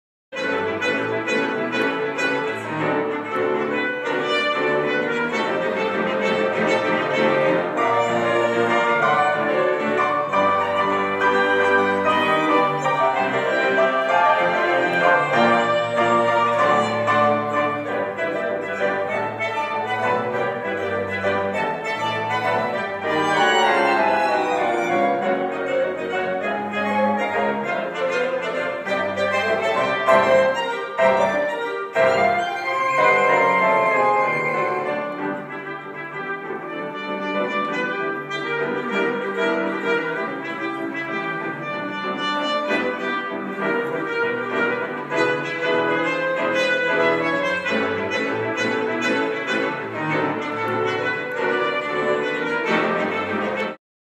Chamber Kicks in Golden Sunday Aug 12, 2012
The Columbia Valley Chamber Music Festival 2012 debuts at the Golden Civic Centre.